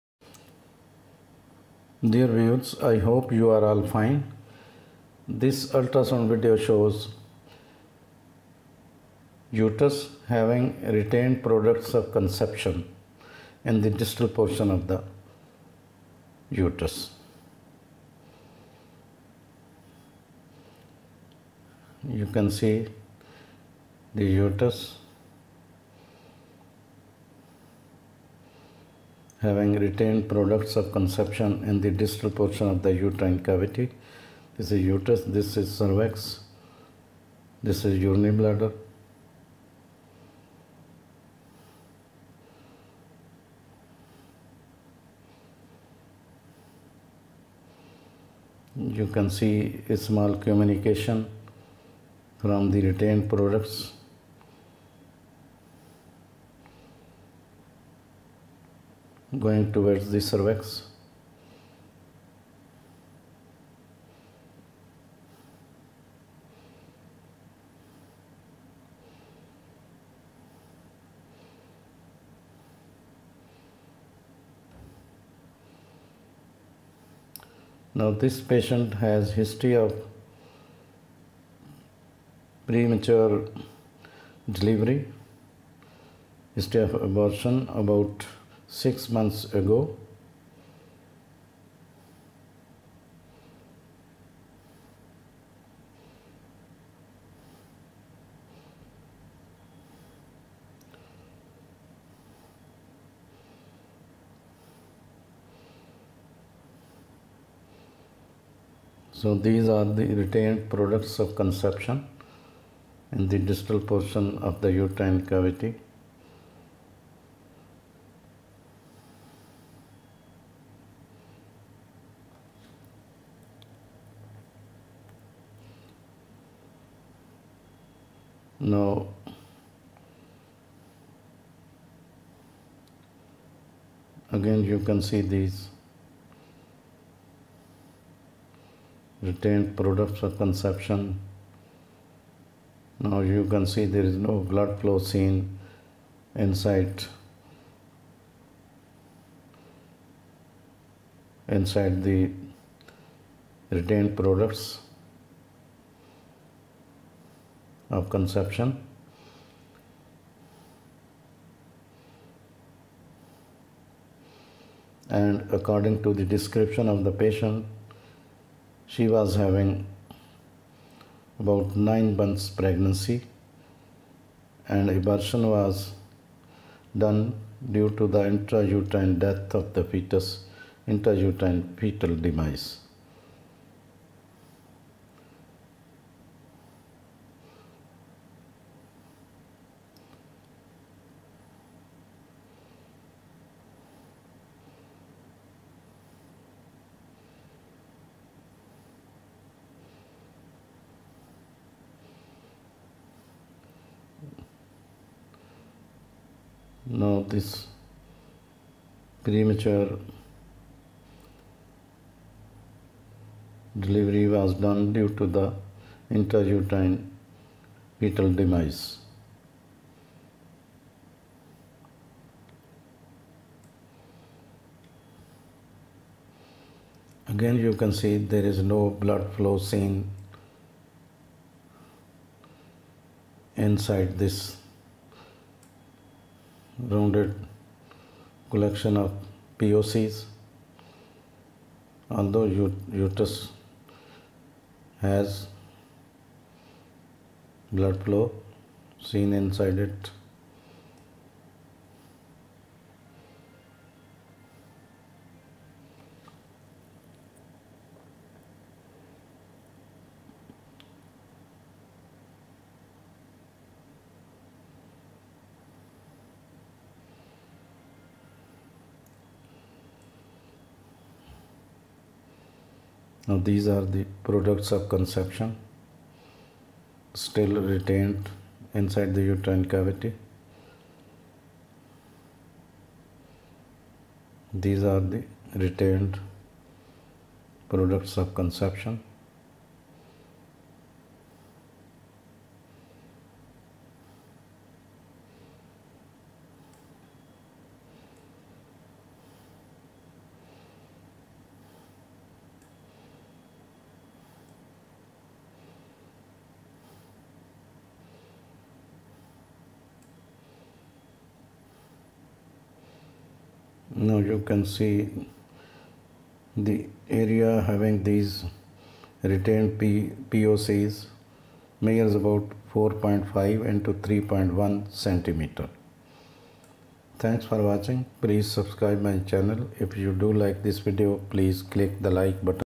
Ultrasound Video showing retained Products of conception inside the uterine cavity.
This video shows retained Products of conception inside the uterine cavity.